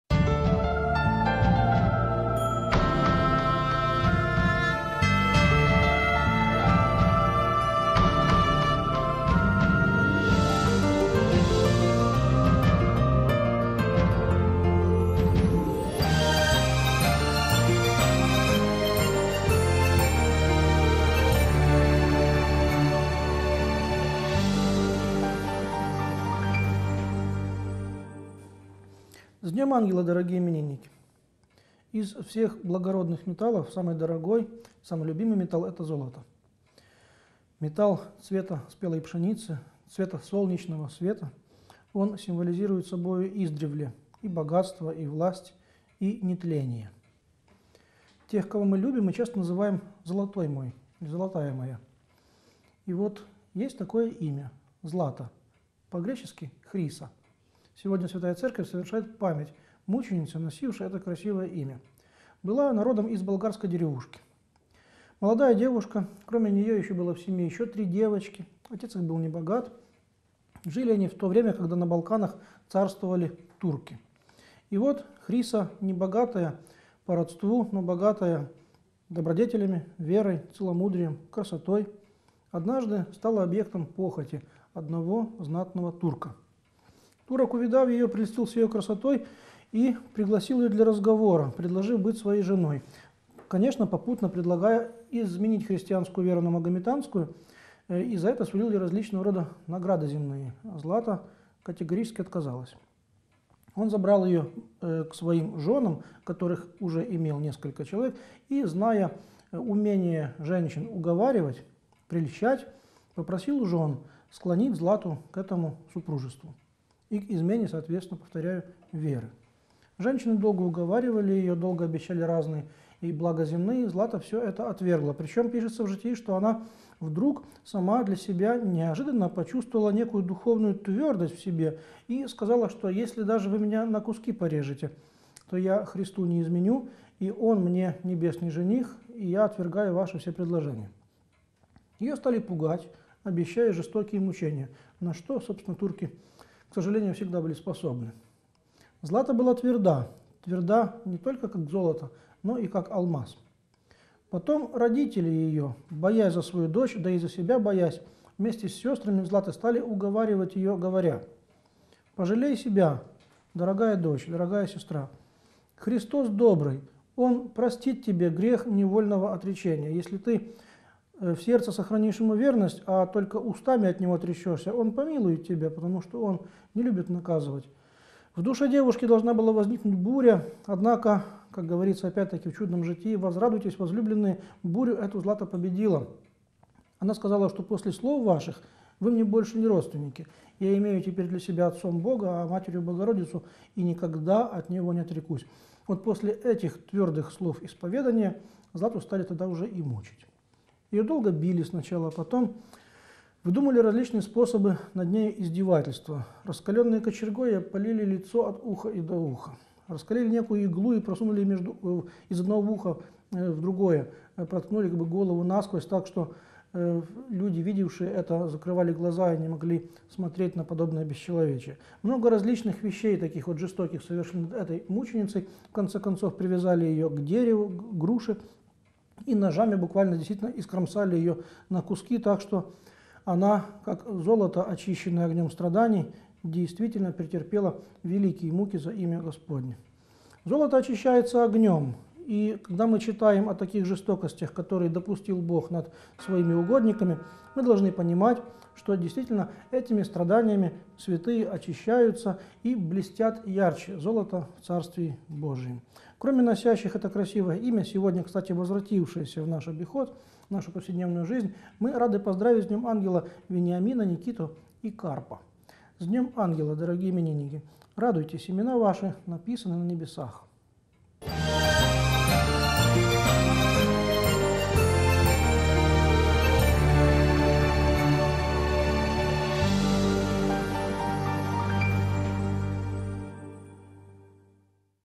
День ангела 26 октября – День преподобного Вениамина Печерского – отец Андрей Ткачёв
26 октября именины отмечают Злата, Антон, Вениамин, Никита, Николай, Трофим, Яков. В этот день церковь почитает память преподобного Вениамина Печерского.

Распространи это видео!